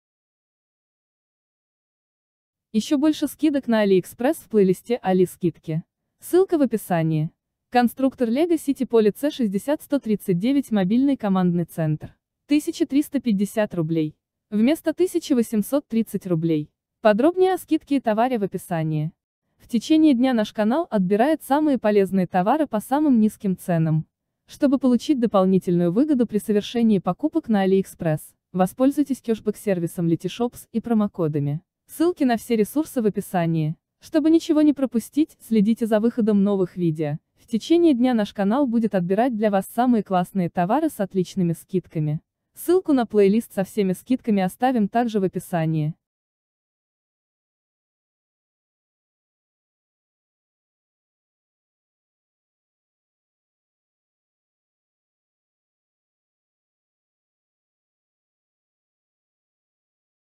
Конструктор LEGO City Police 60139 Мобильный командный центр | скидки на Али
Конструктор LEGO City Police 60139 Мобильный командный центр
Вдруг ещё кто-то от lego не устал
Цену также можно сбить монетами (-10%)

Кешбек сервис LetyShop (скидка до 80% в более 1500 магазинов, в том числе Aliexpress.)